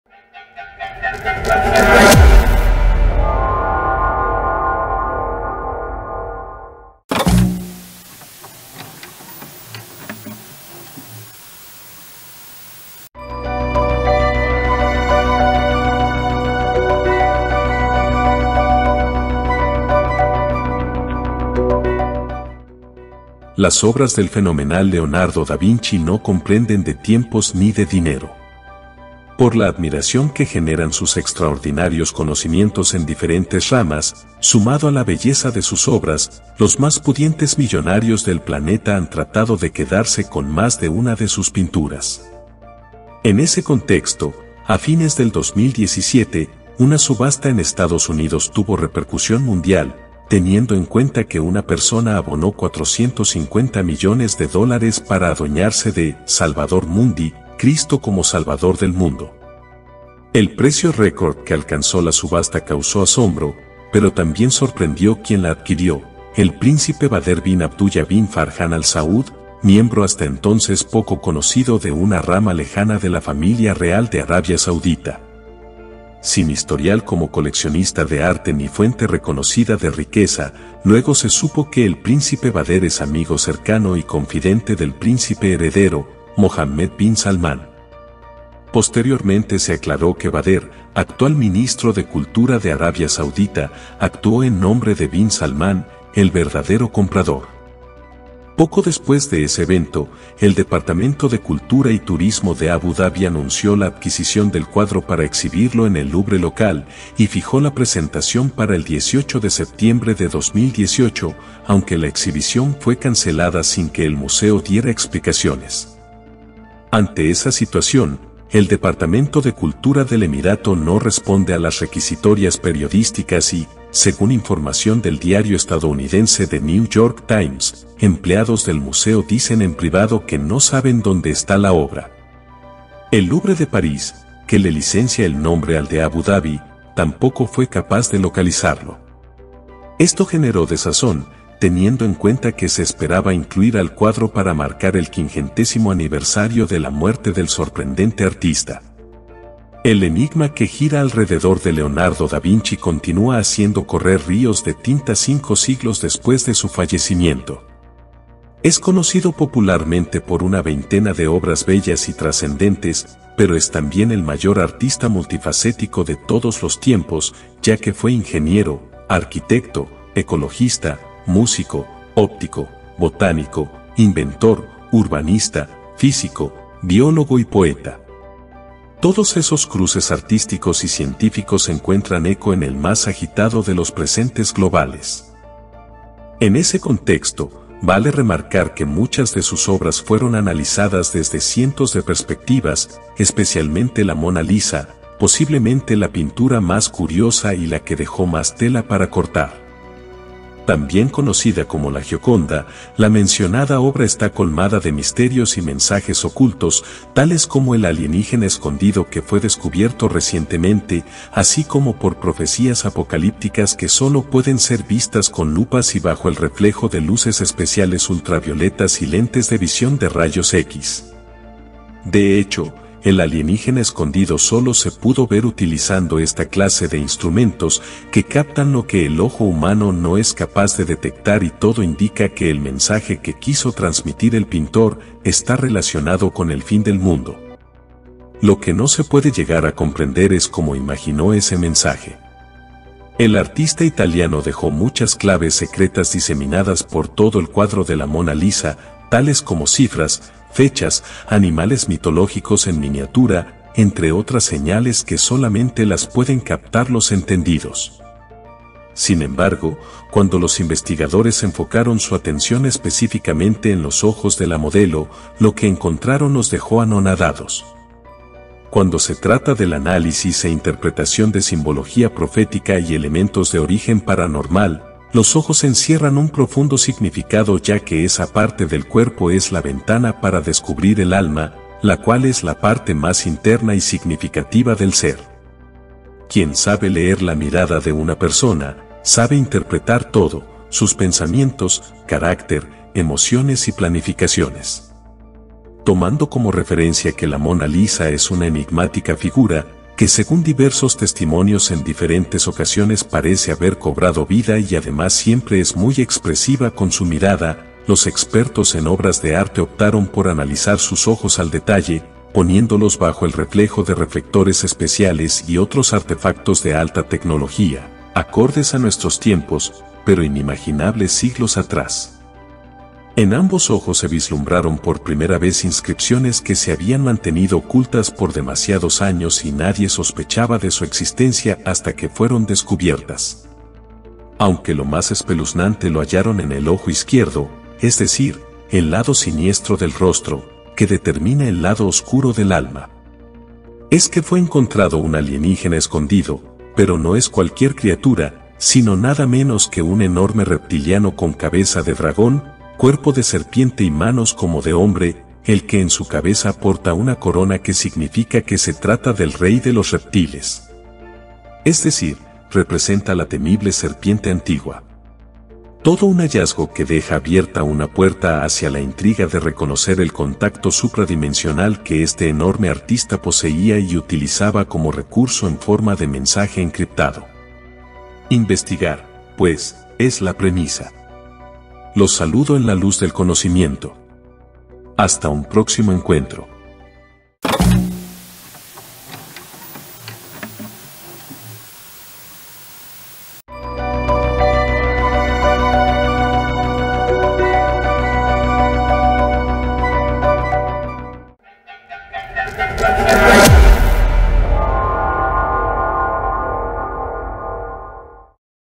TECNOLOGÍA 5D * PINTURAS y MISTERIOS * JULIO ARCHET
Las obras del fenomenal Leonardo no comprenden de tiempos ni de dinero. Por la admiración que generan sus extraordinarios conocimientos en diferentes ramas, sumado a la belleza de sus obras, los más pudientes millonarios del planeta han tratado de quedarse con más de una de sus pinturas. En ese contexto, a fines del 2017, una subasta en Estados Unidos tuvo repercusión mundial, teniendo en cuenta que una persona abonó 450 millones de dólares para adueñarse de “Salvador Mundi” (Cristo como salvador del mundo).